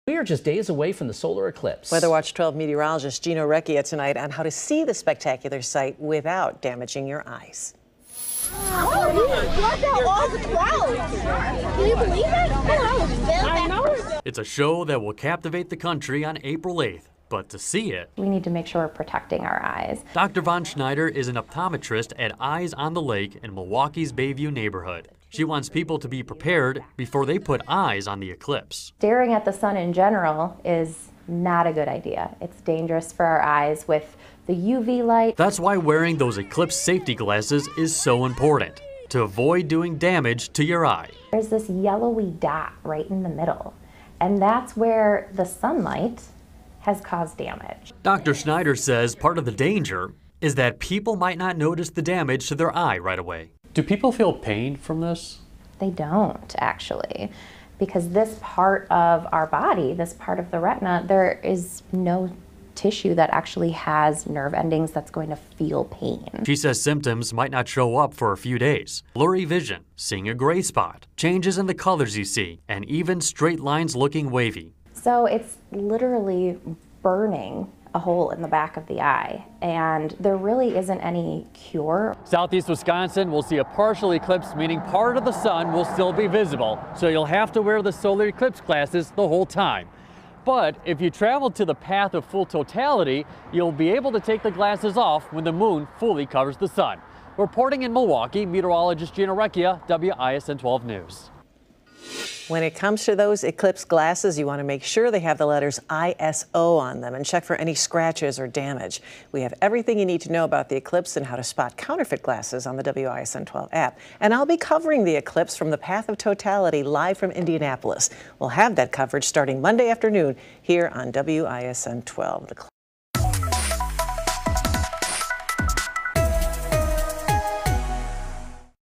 How to protect your eyes for the solar eclipse
To protect your eyes, those eclipse safety glasses need the letters ‘ISO’ on them and be sure they are not scratched or damaged.